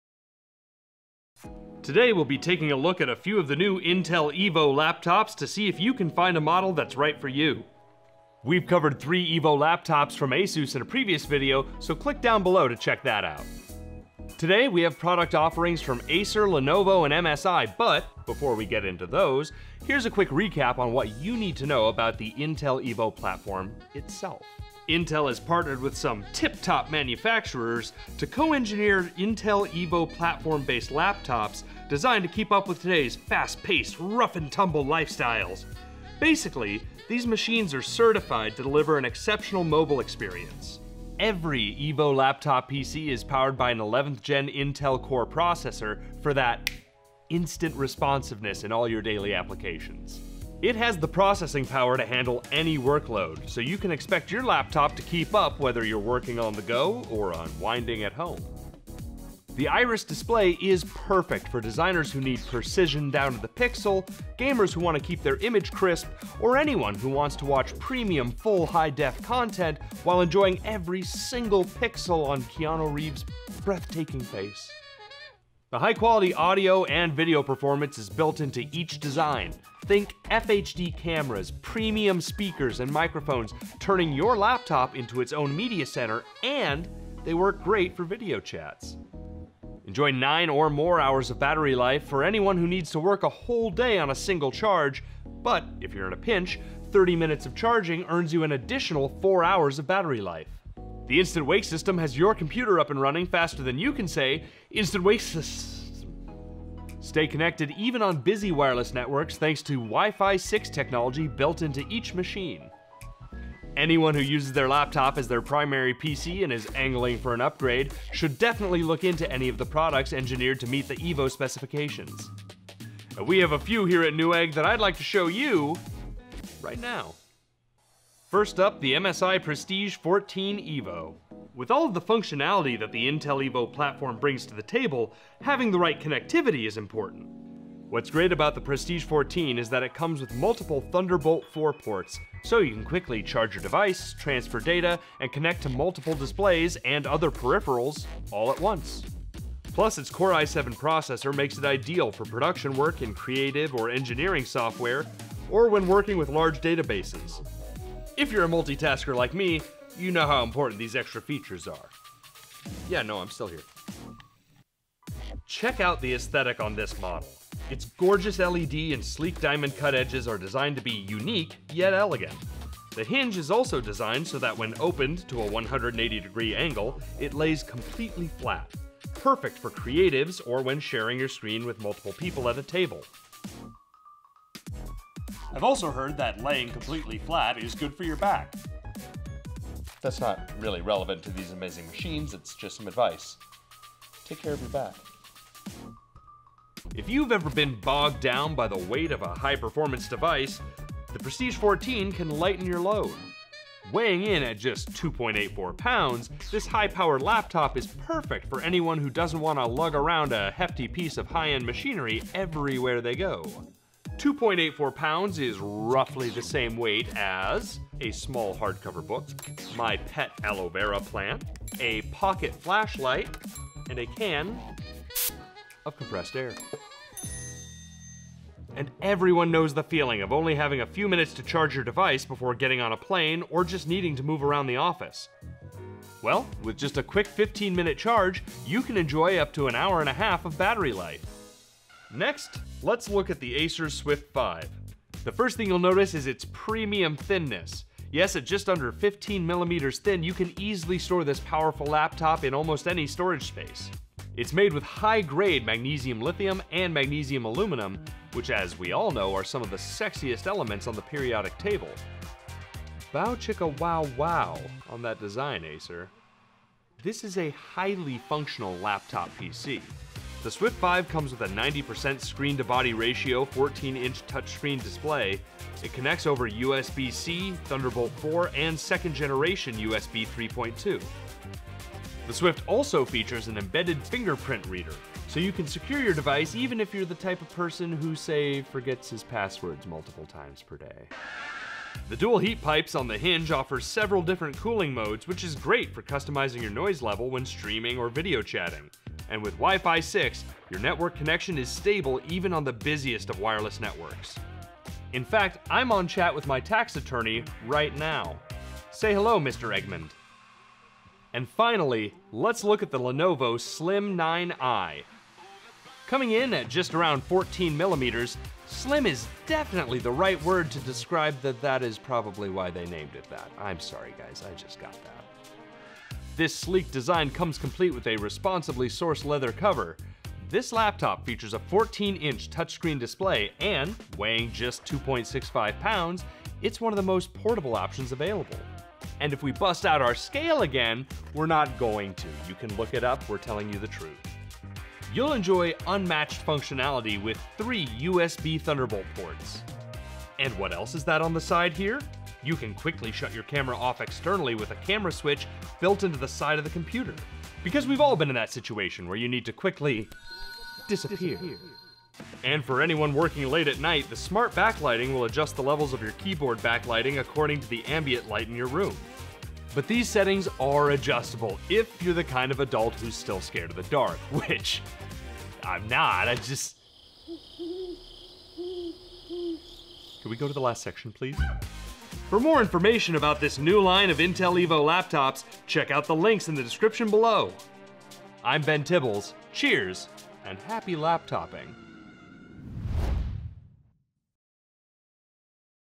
Introducing Intel® Evo™ Laptops by Acer, MSI, and Lenovo
The new Intel EVO platform is made up of laptops that meet certain standards based on usability in real world situations. These requirements include “Key Experience Indicators” (KEI) like battery life, charging speed, and system responsiveness, as well as specifications like Wi-Fi 6 compatibility, and 11th Gen Intel Core i5 or i7 CPU with Intel Iris Xe integrated graphics.

We're got three EVO platform Windows laptop PCs deck to show you today from MSI, Acer, and Lenovo.

What’s great about the Prestige 14 is that it comes with multiple Thunderbolt 4 ports so you can quickly charge your device, transfer data, and connect to multiple displays and other peripherals all at once. Plus, its Core i7 processor makes it ideal for production work in creative or engineering software.

The Swift 5 has a 90 percent screen-to-body ratio 14-inch touch screen display. It connects over USB-C, Thunderbolt 4, and second generation USB 3.2 (usb 3.2 gen 2).
It also comes with an embedded fingerprint reader, allowing you to secure your device without needing to remember your password multiple times per day.

Coming in at just around 14 millimeters, Slim is definitely the most accurate word to describe this laptop. You’ll enjoy unmatched functionality with 3 USB Thunderbolt ports. The sleek design comes complete with a responsibly sourced leather cover.

This content was created in cooperation with Intel.